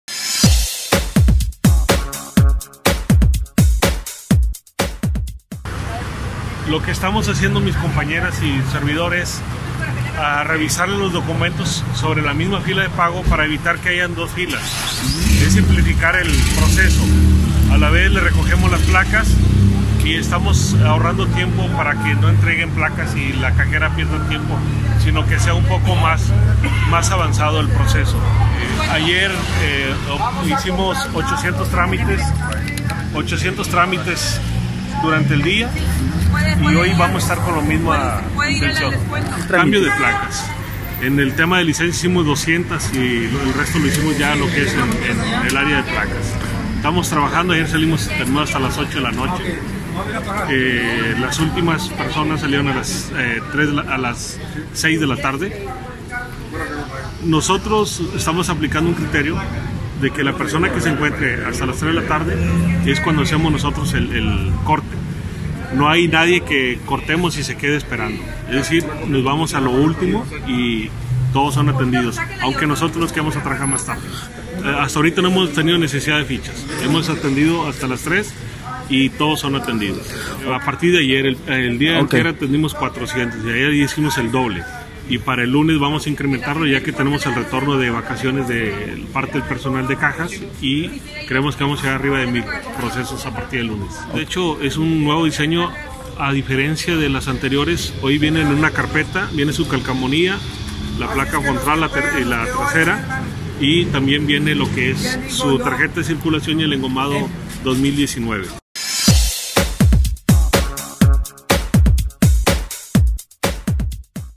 Pago de placas en la Oficina Fiscal
Alrededor de 800 personas son atendidas en la Oficina Fiscal del Estado por concepto del trámite de cambio de placa